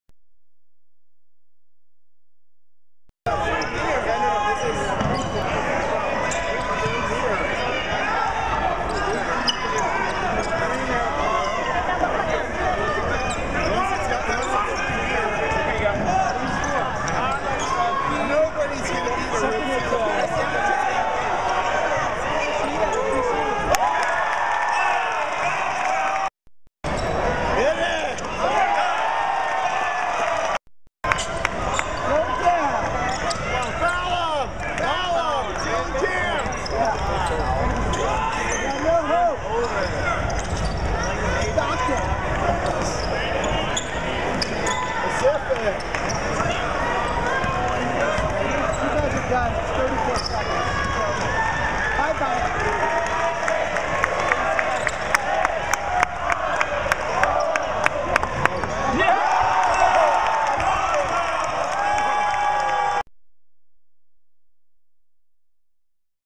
Kyle Boswell, 2-9-13
UCSB's Kyle Boswell highlights vs. UOP in the Thunderdome.